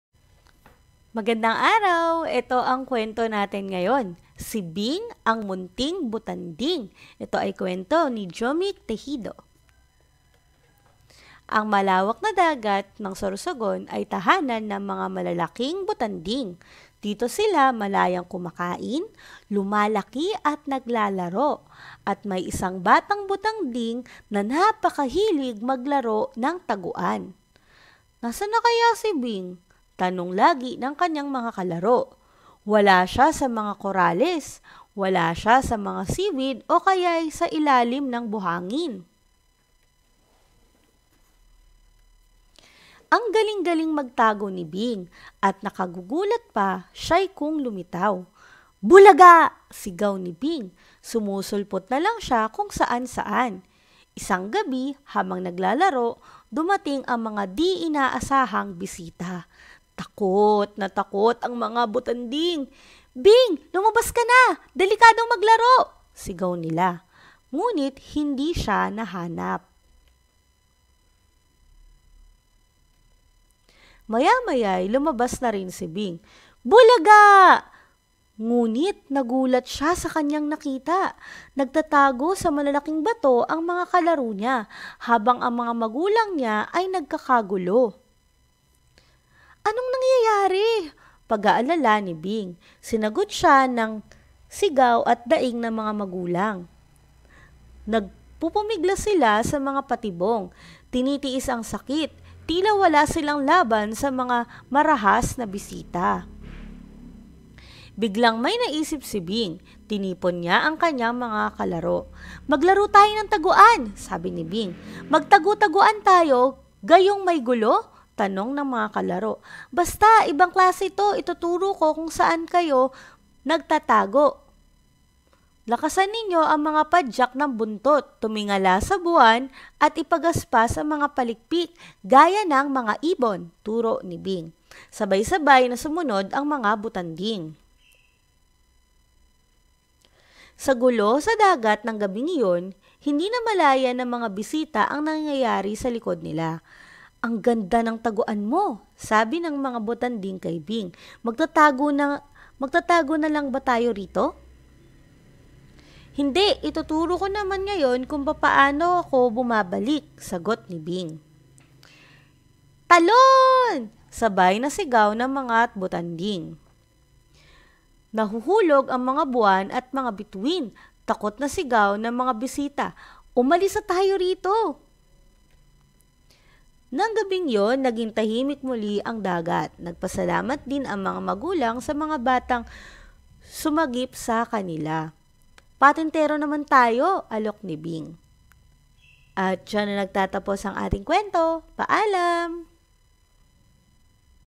Kwentong Pambata - Si Bing ang Munting Butanding
Ang kwentong ito ay tungkol sa butanding na maliksi at kung papaano niya niligtas ang kanilang sarili sa masamang bisita.